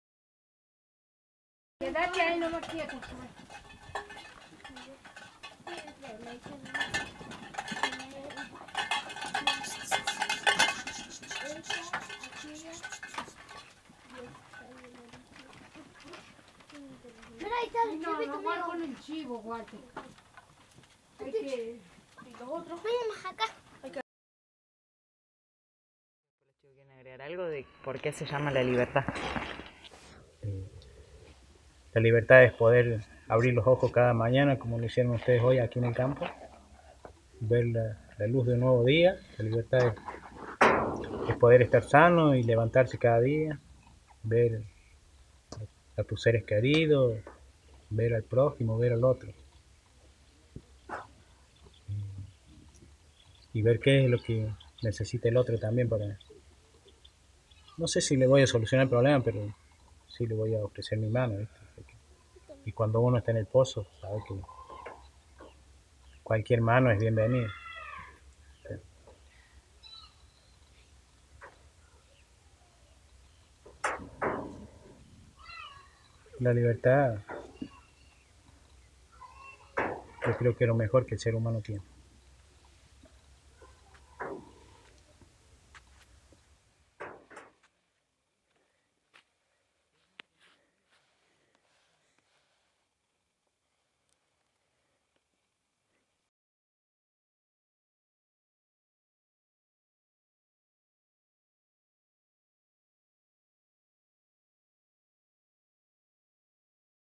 Habitar La Libertad - Movimiento Campesino de Córdoba
Un recorrido por la comunidad campesina que desde hace décadas convive en un territorio de 13 mil hectáreas en las puertas de las Salinas Grandes. Un día con mujeres, hombres y niñxs que hacen del campo un lugar donde vivir. Un encuentro con treinta familias que hoy enfrentan, una vez más, el intento de rematarles una parte de su tierra y su libertad.